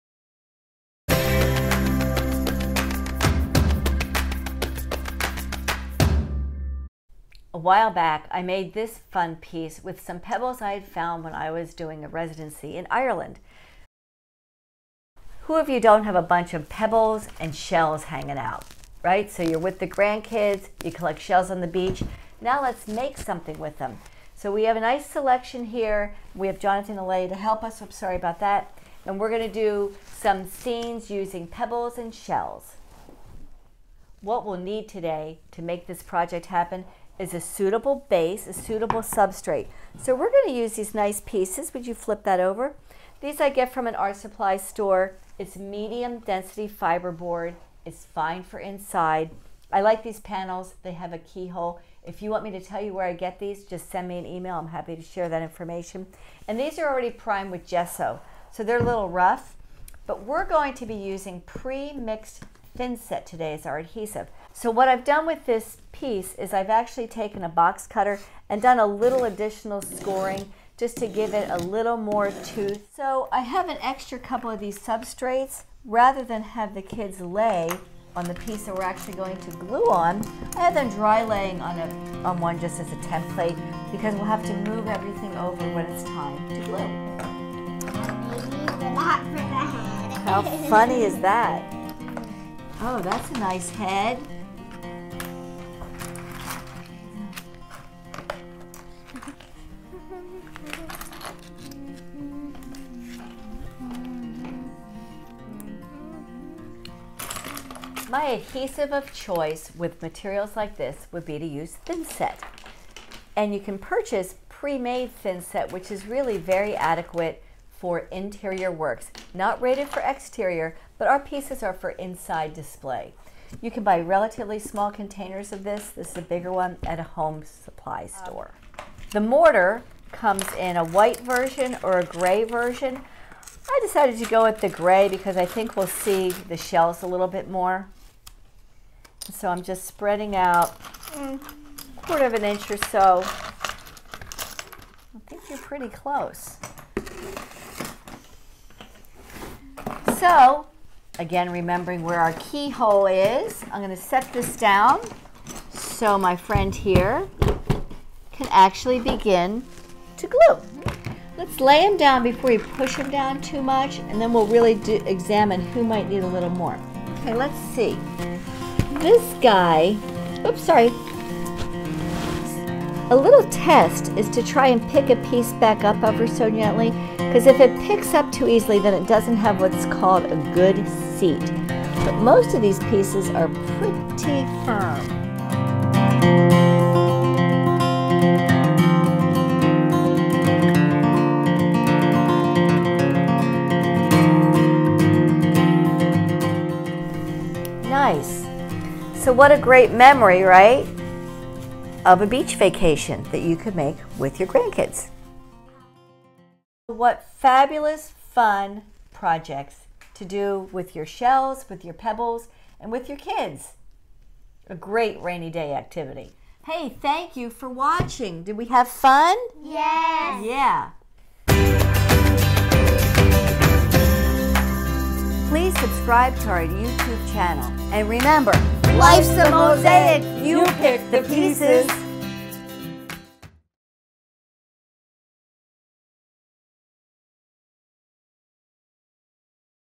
DIY Pebble and Shell Mosaic Art Project with Kids
Discover an Entertaining DIY: Pebble and Shell Mosaic Art for Kids! Watch 'Make it Mosaics' guide you through crafting a vibrant mosaic masterpiece using shells and pebbles, ideal for a family bonding activity. Embrace your collected treasures from nature walks and vacations in this simple, no-cutting-required project. Materials include wood as a stable base, pre-mixed thin-set mortar, and your cherished shells and pebbles. Get creative with your kids and embark on this fun artistic journey!

Helpful Hint: Wash and allow to dry shells and pebbles in advance, you will get better adhesion.

Great Books: 300+ Mosaic Tips
Guide to Mosaic Techniques

Other GOOD project boards
SimpleSet Pre-Mixed Thin-Set Mortar